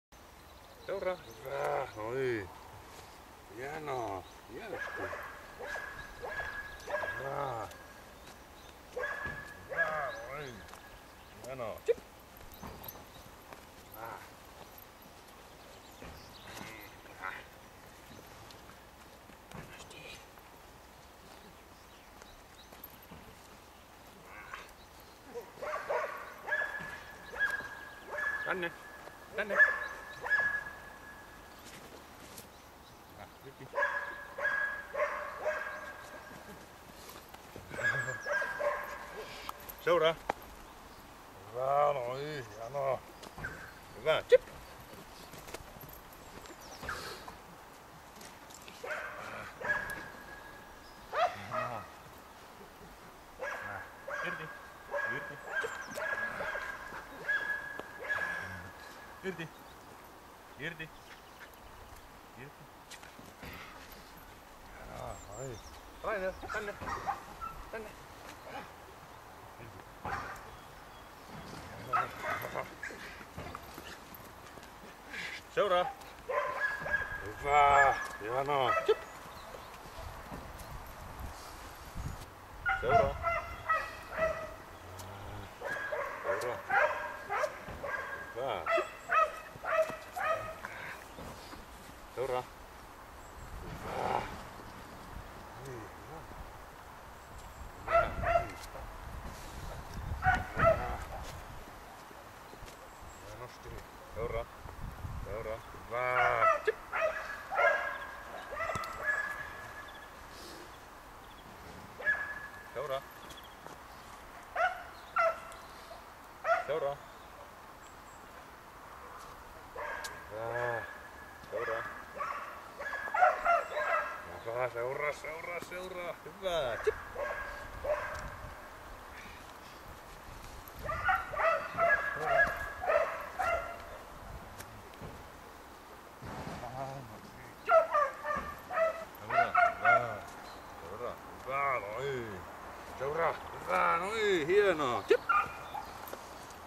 UkRainer iz Gratsiano 4 months old obedience
Malinois puppy obedince with toy and food
handler Jan Skogster